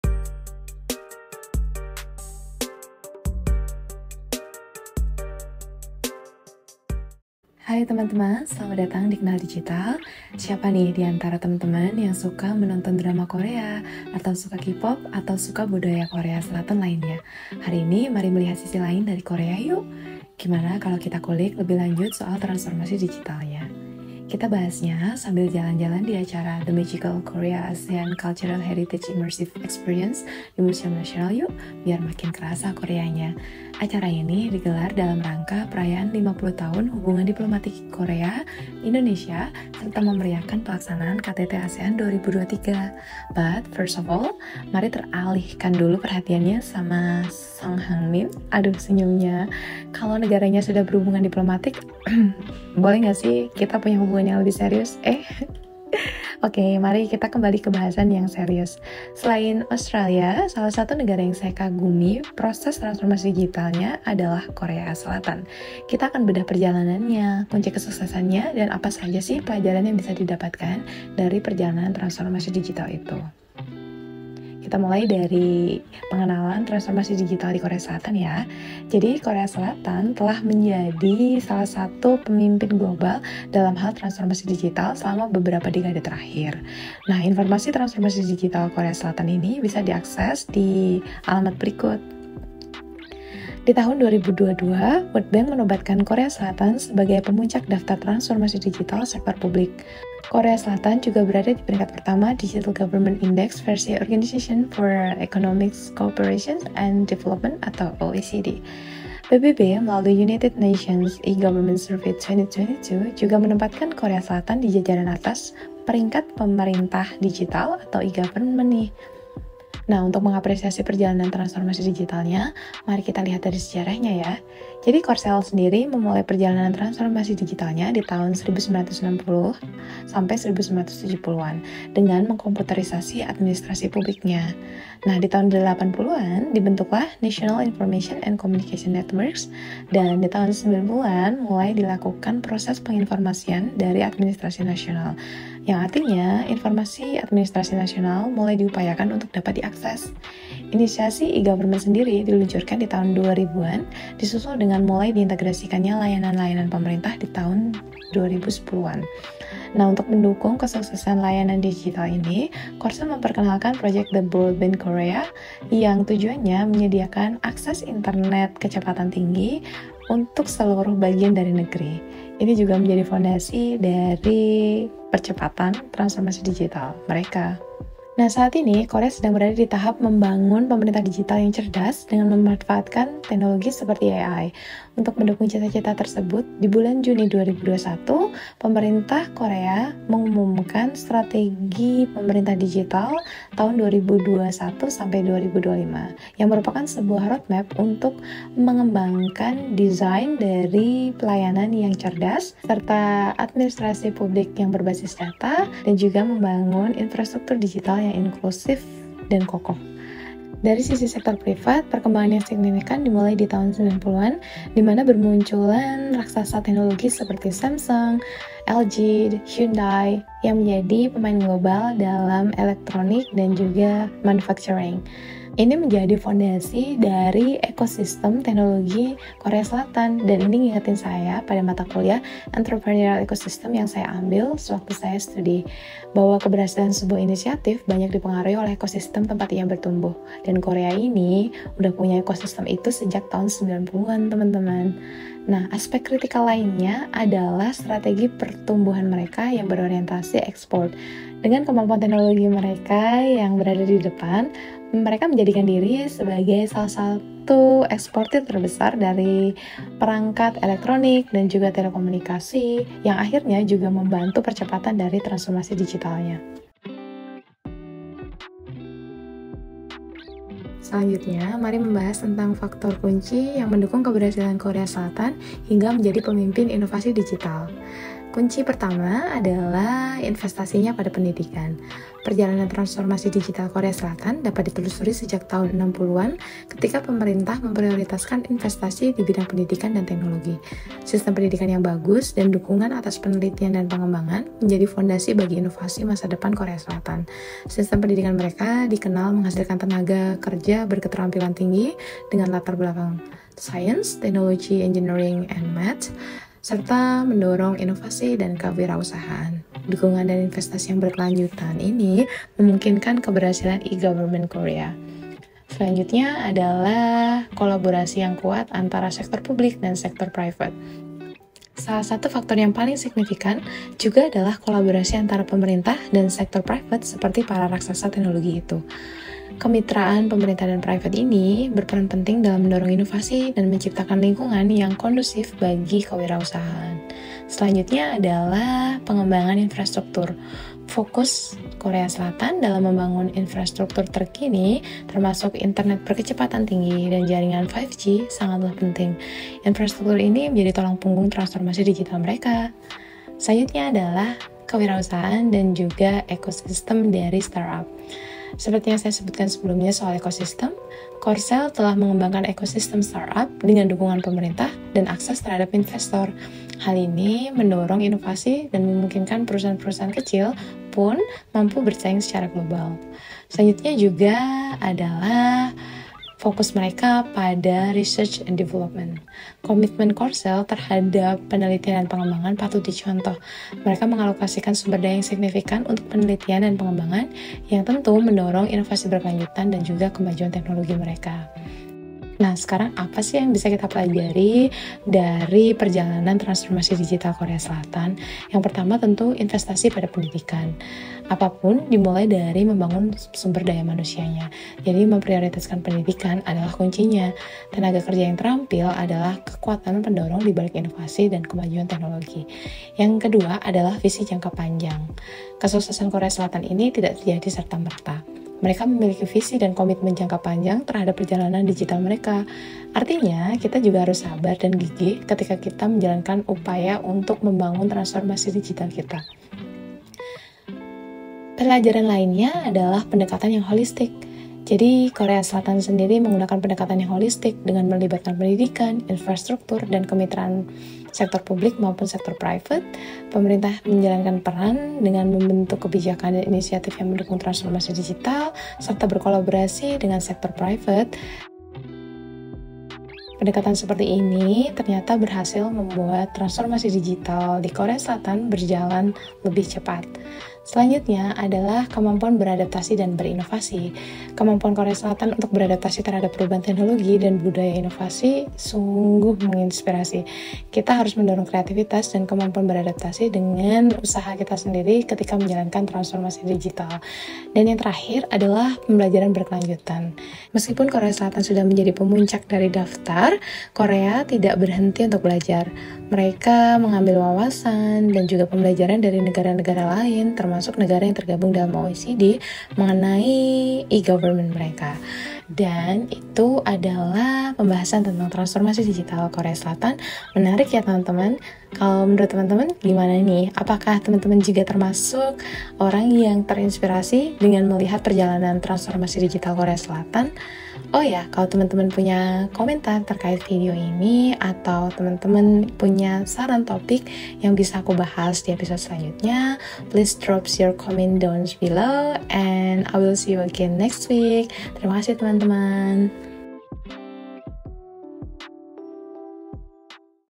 Belajar Dari Sang Juara Transformasi Digital | Kenal Digital Episode 8
Selamat datang di Kenal Digital, sebuah program untuk berbagi wawasan terkait transformasi digital dengan host Nur Imroatun Sholihat (iim). Di episode ini, iim membahas perjalanan transformasi digital Korea Selatan. Selamat menyaksikan.

Sampai bertemu di episode selanjutnya setiap senin pukul 09.00 WIB ya.